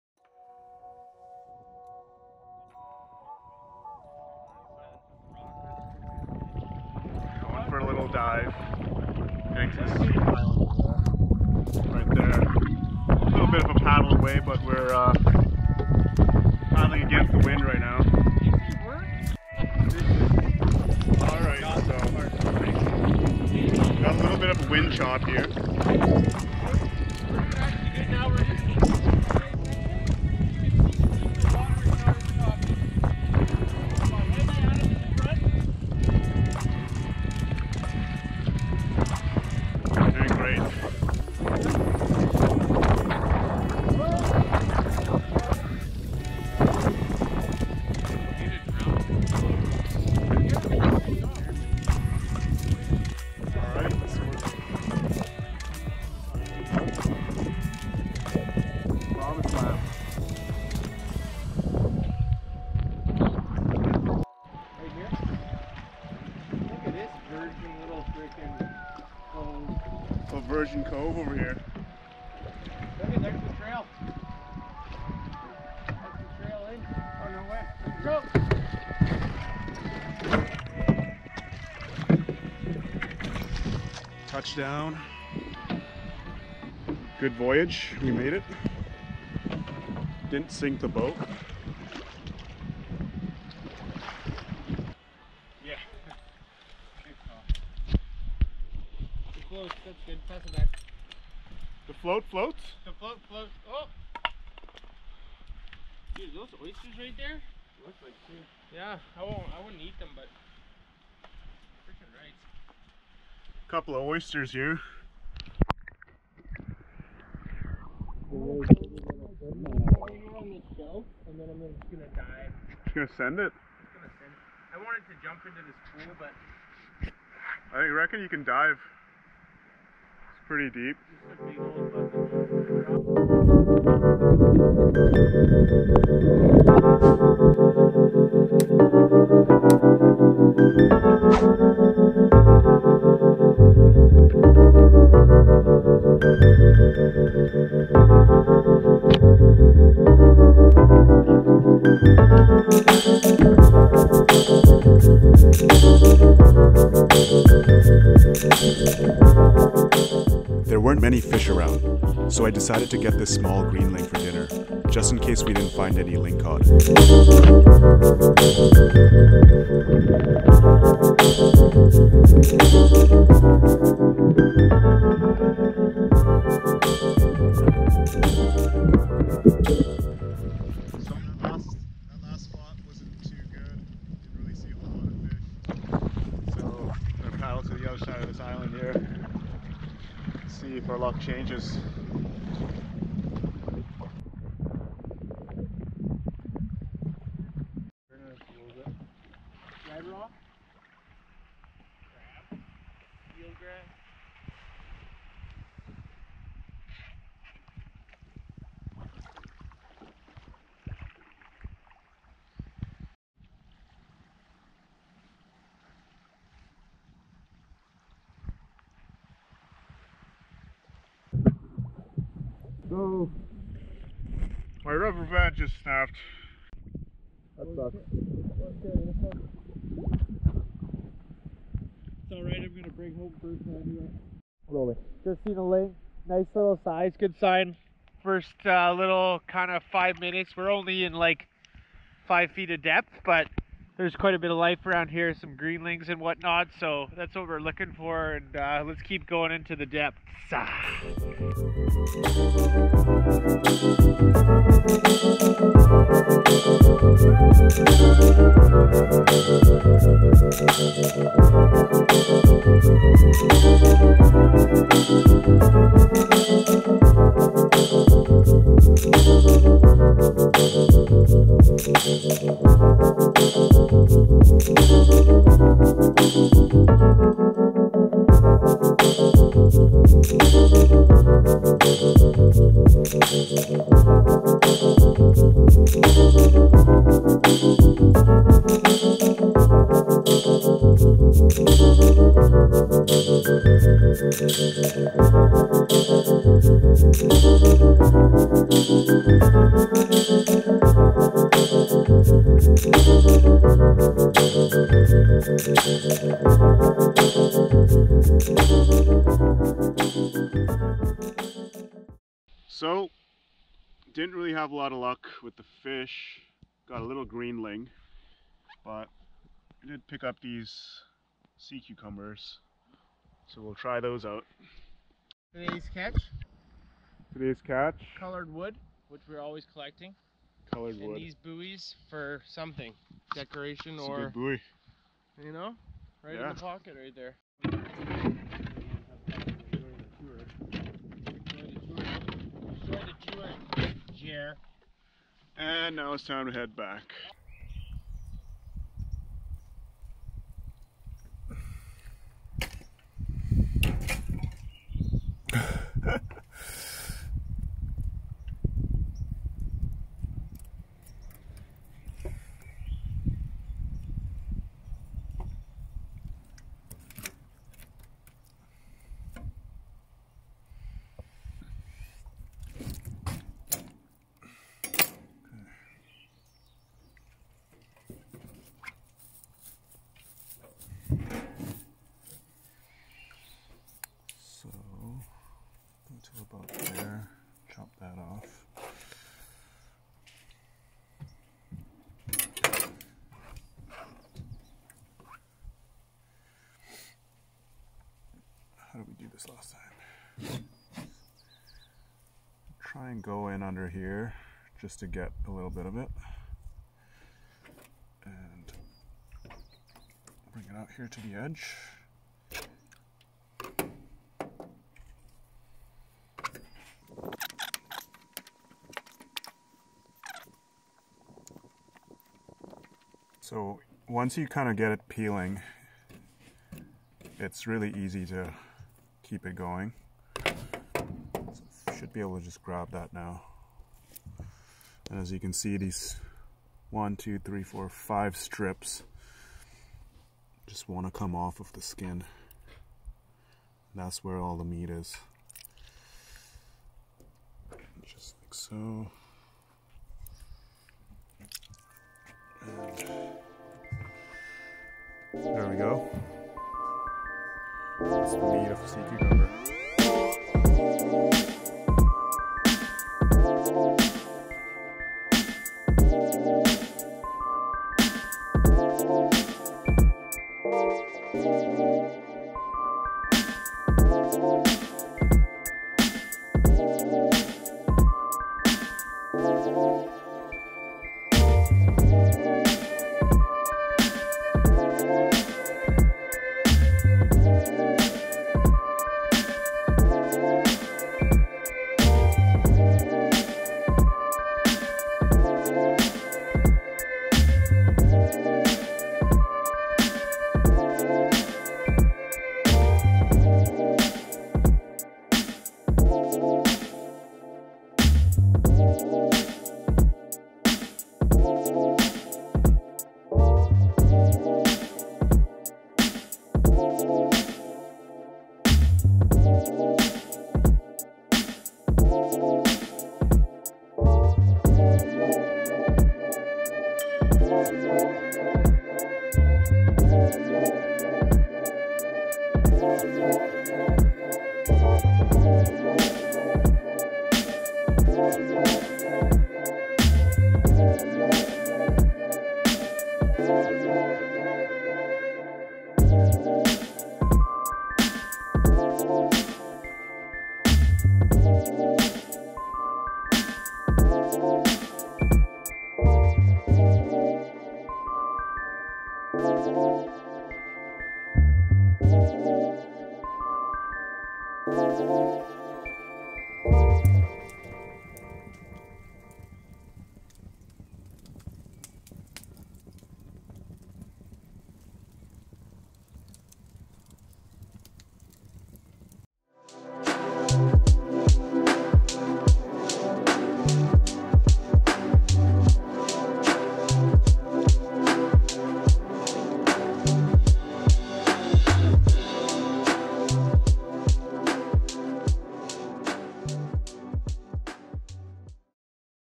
FREEDIVING for SEA CUCUMBER (Catch Clean Cook) - Spearfishing Vancouver Island BC - 4K
We went on a canoe adventure in search of a good spot to freedive for some dinner. The spearfishing was below average that day, with no good lingcod spotted, but lucky for us the ocean around Vancouver Island is rich with other types of edible species. Sea Cucumber is not an ingredient that I am very familiar with, but we tried to make a Ceviche with it. It turned out to be quite delicious, and even the cleaning process is pretty straight forward once you know what to do.  Hope you guys enjoyed the video! :)